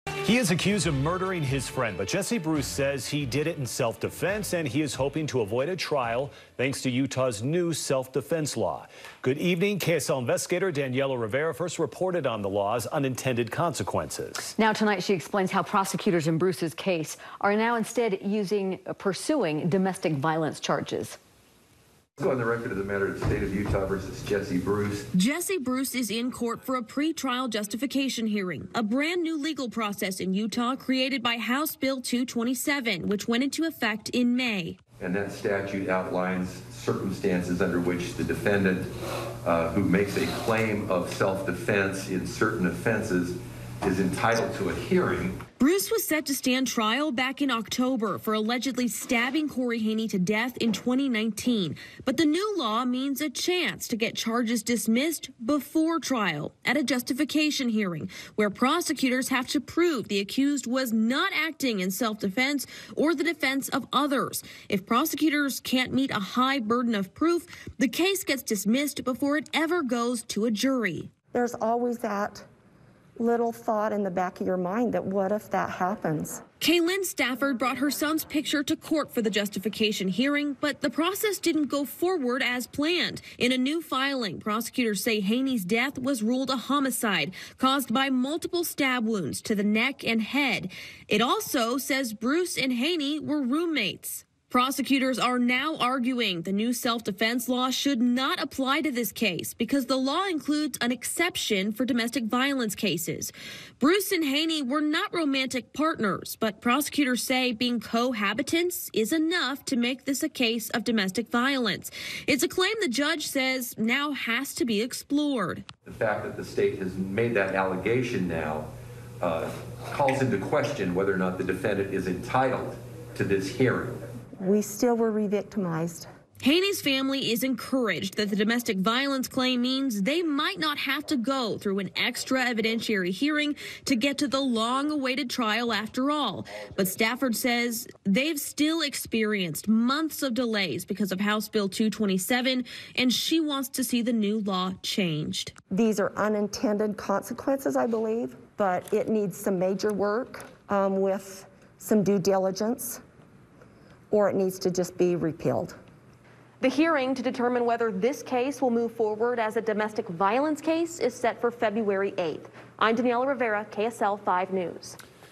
Prosecutors hope to circumvent Utah’s new, controversial self-defense law in 2019 murder case
A new claim that the evidence in a 2019 murder case constitutes domestic violence could spare prosecutors from a new hearing process that creates an opportunity for the defendant’s charges to be dismissed before trial.